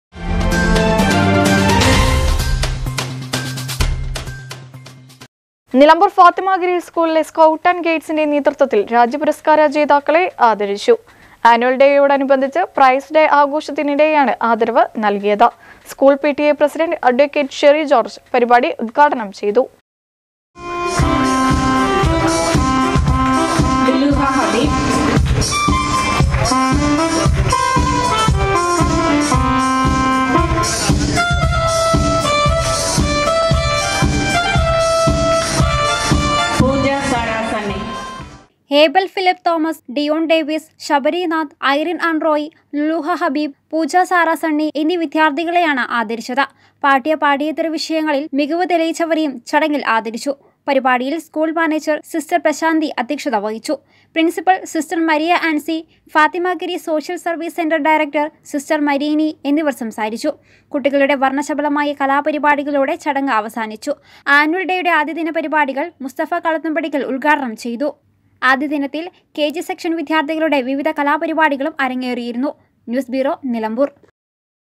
ONE TV NEWS
നിലമ്പൂര്‍ ഫാത്തിമാഗിരി സ്‌കൂളില്‍ സ്‌കൗട്ട് ആന്റ് ഗൈഡ്‌സിന്റെ നേതൃത്വത്തില്‍ രാജ്യപുരസ്‌കാര ജേതാക്കളെ ആദരിച്ചു. ആനുവല്‍ ഡേയോടനുബന്ധിച്ച് പ്രൈസ്‌ഡേ ആഘോഷത്തിനിടെയാണ് ആദരവ് നല്‍കിയത്. സ്‌കൂള്‍ പി.ടി.എ. പ്രസിഡന്റ് അഡ്വ. ഷെറി ജോര്‍ജ് പരിപാടി  ഉദ്ഘാടനം ചെയ്തു.